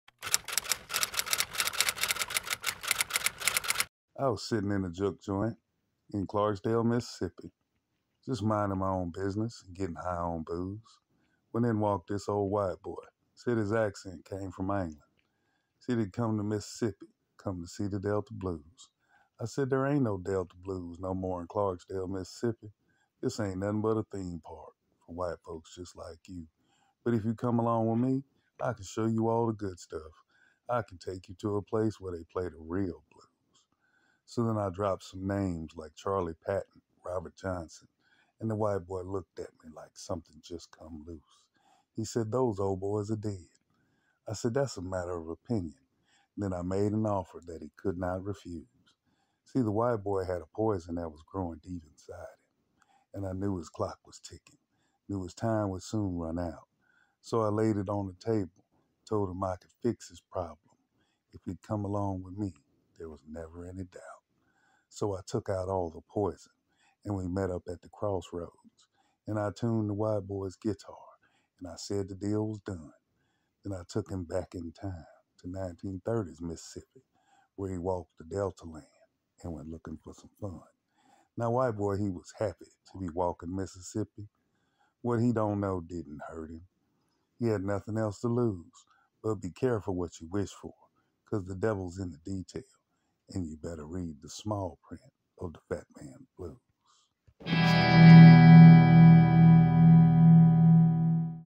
Fat Man Blues by Richard Wall, read by Michael Clark
This is a poem I wrote based on my novel, Fat Man Blues, which is available from:

Michael Clark's writing can be found on michaelclarkwrites.blogspot.com